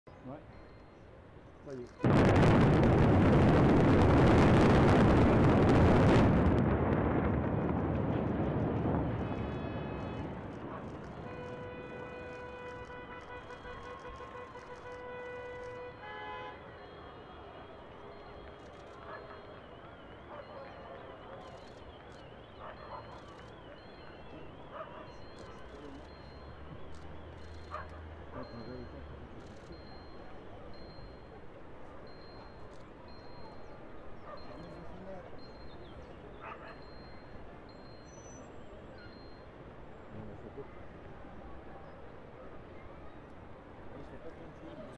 France : impressionnante destruction d'un HLM dans la région lyonnaise
En moins de 5 secondes, la barre "Monmousseau" qui se dressait depuis 1967 à Vénissieux, dans la banlieue de Lyon, a été réduite en un gigantesque tas de gravas et de poussières pour laisser la place à un chantier de rénovation urbaine.